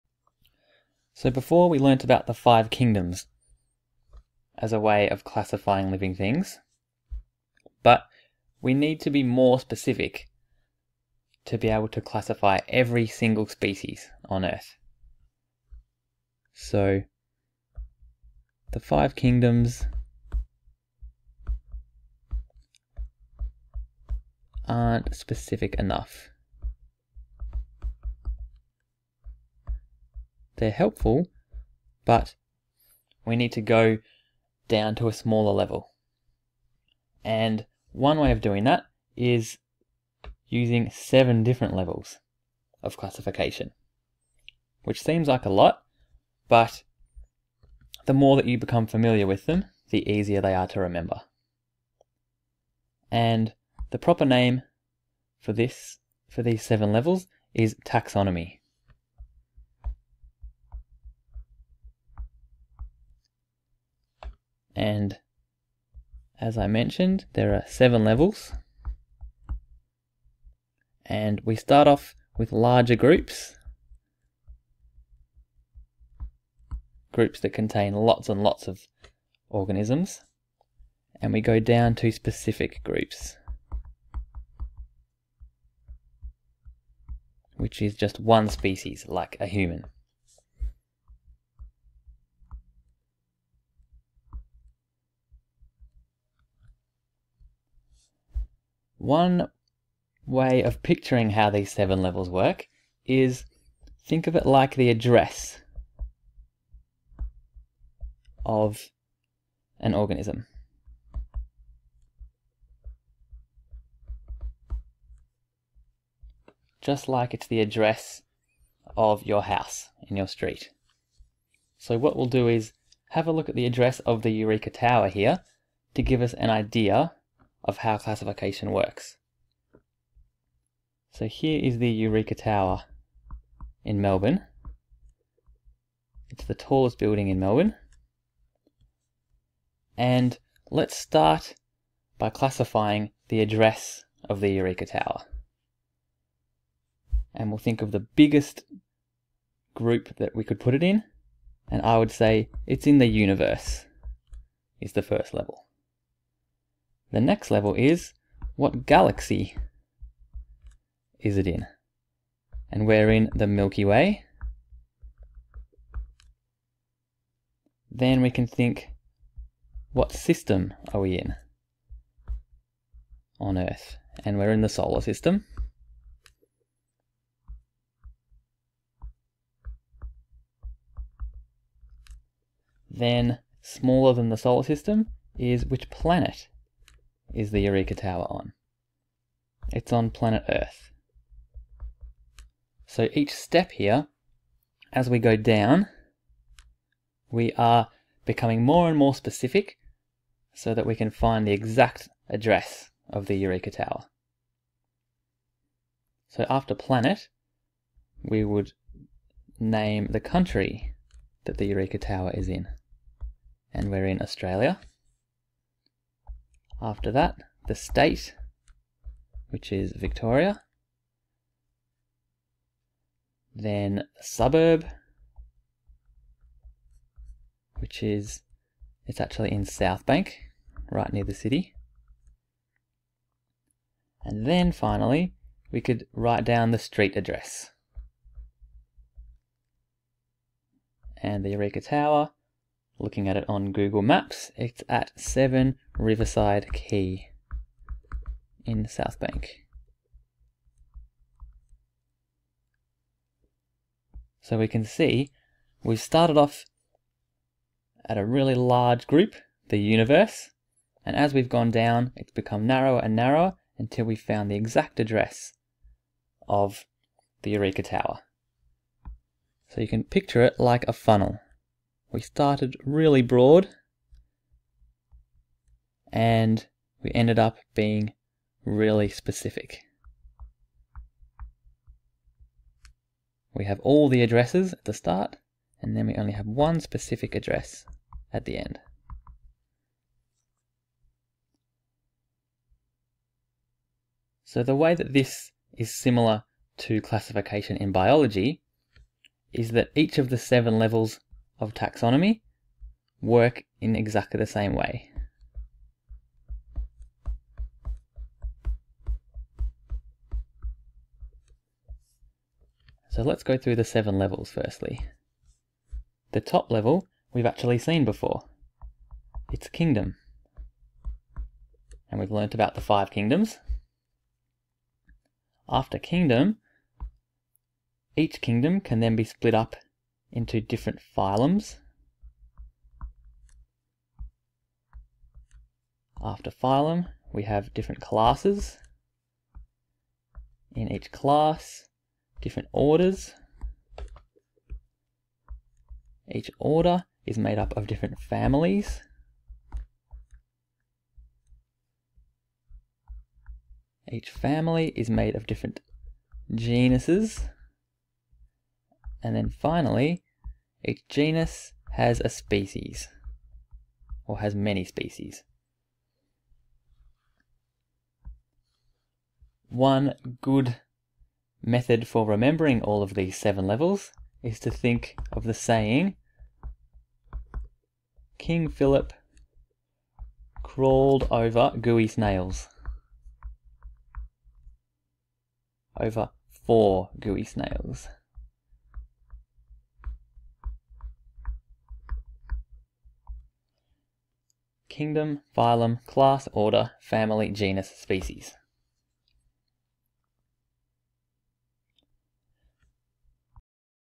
9 - The Levels of Taxonomy
Questions:
1) Define ‘taxonomy’
2) List the seven levels of taxonomy